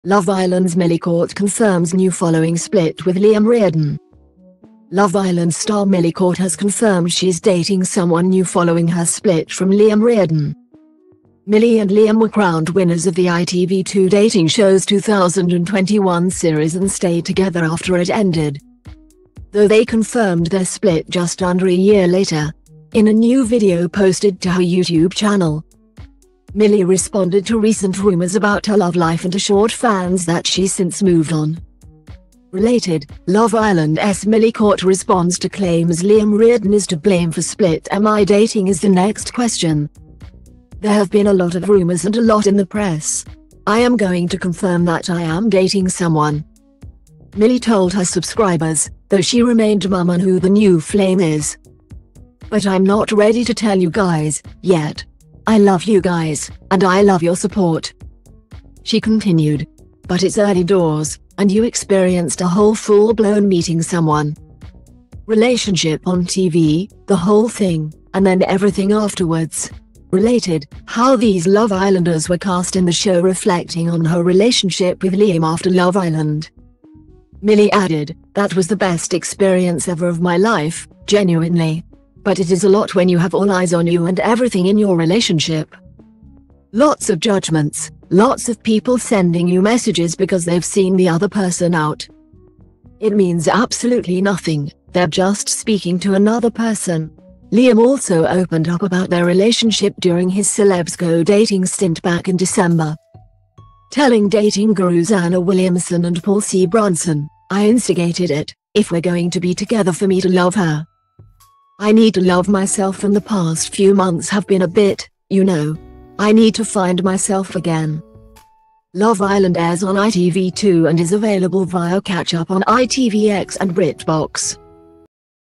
Love Island's Millie Court confirms new following split with Liam Reardon
Love Island star Millie Court has confirmed she's dating someone new following her split from Liam Reardon .Millie and Liam were crowned winners of the ITV2 dating show's 2021 series and stayed together after it ended,though they confirmed their split just under a year later. In a new video posted to her YouTube channel,